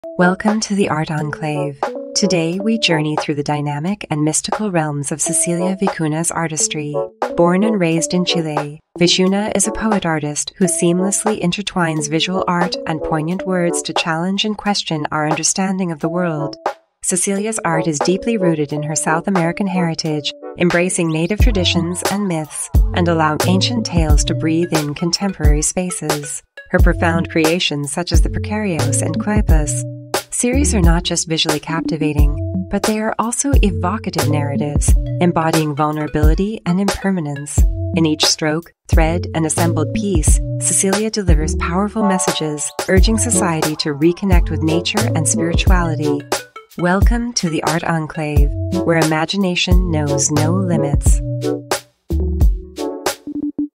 Cecilia Vicuña: Weaving Ancestral Echoes into Contemporary Art
Dive into the mesmerizing world of Cecilia Vicuña, a pioneering artist who seamlessly blends Chilean traditions with contemporary themes. Discover her profound connection to indigenous heritage and witness how she channels it into groundbreaking artwork. A journey from ancestral echoes to modern masterpieces